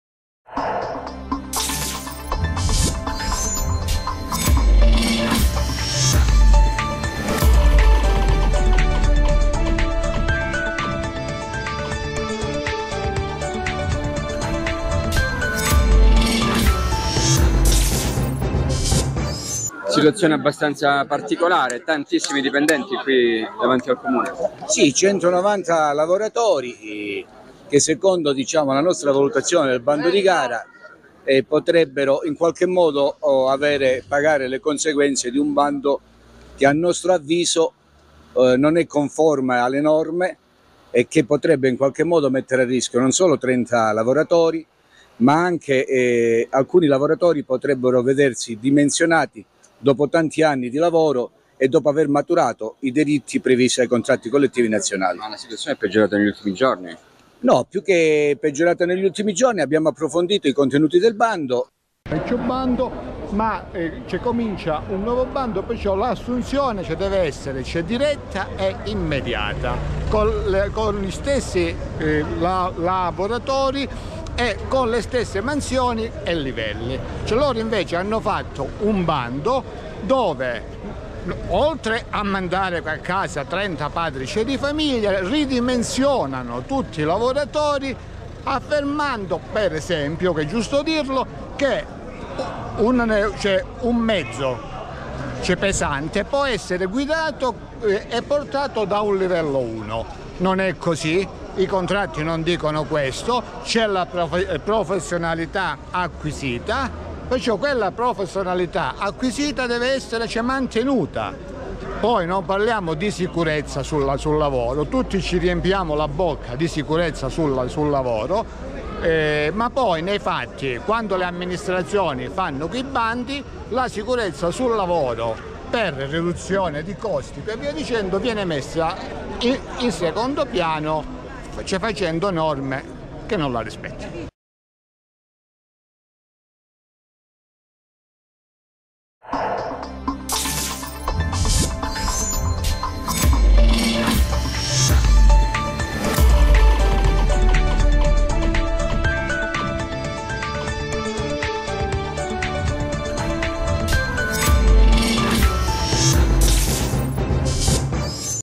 Catanzaro, manifestazione dipendenti Sieco a rischio licenziamento e dimensionamento - RTC
Servizio di Redazione RTC Catanzaro